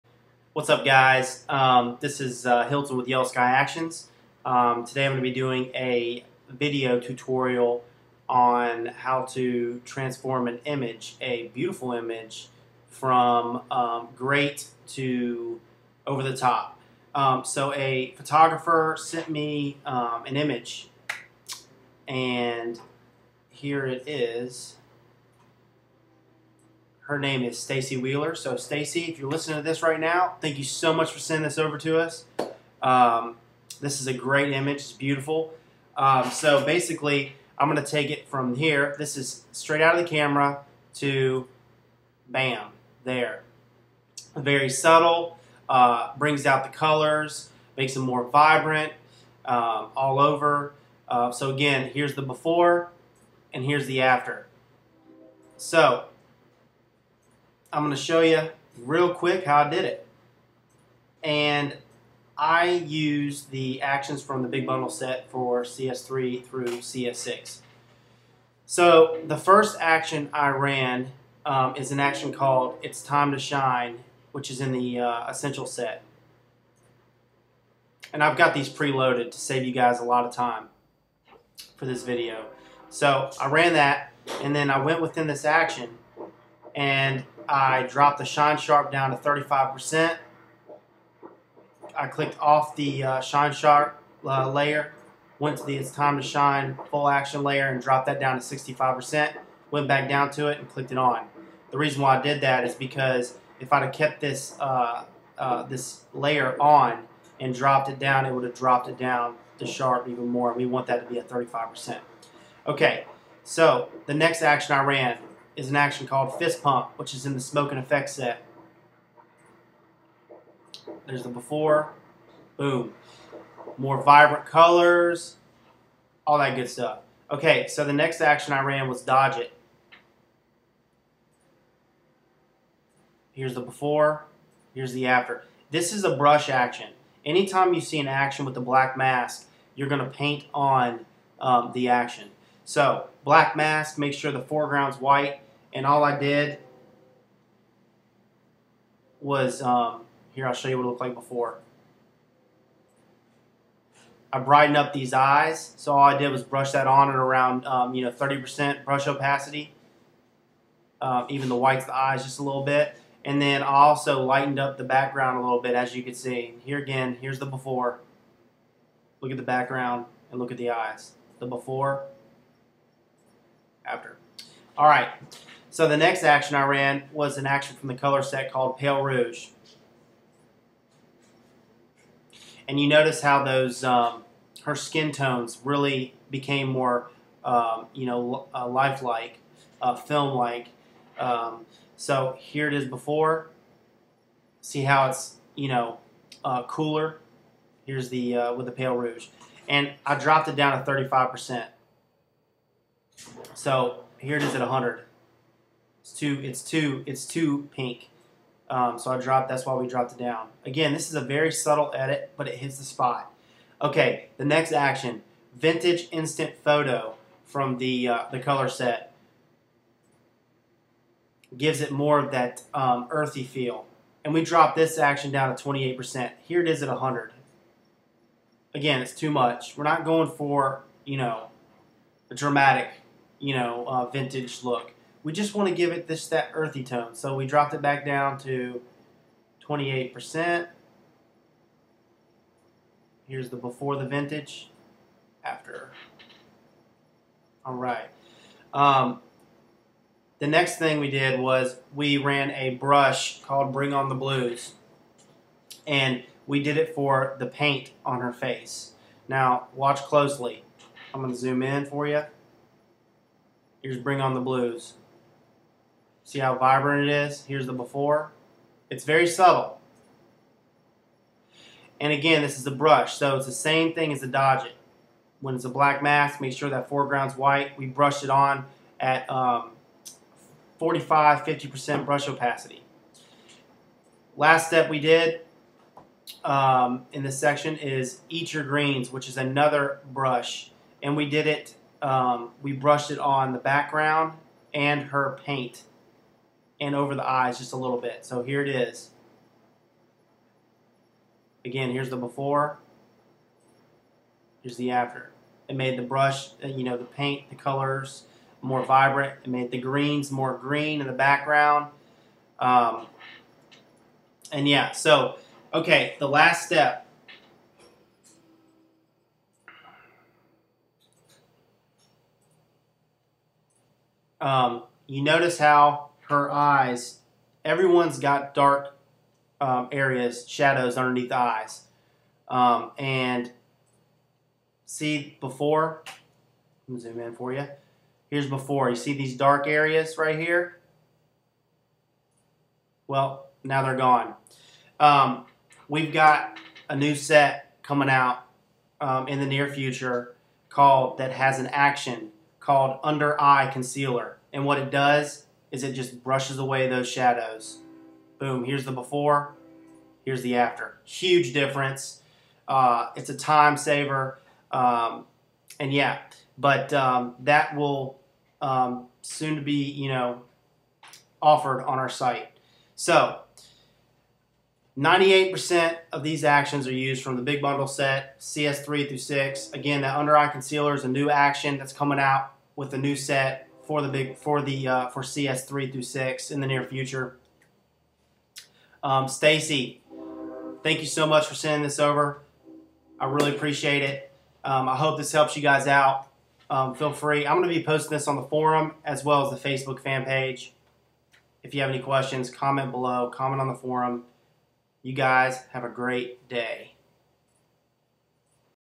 Enhancing Image Vibrancy!
Thank you so much Stacey Wheeler for sending us this beautiful image to work on. This is a video tutorial on how to make and image more vibrant and sharper using our actions from the Big Bundle Set for CS3-6. Below is the recipe used.

Its Time To Shine, Fist Pump, Dodge It, Pale Rouge, Vintage Instant Photo, Bring On The Blue, and Eat Your Greens.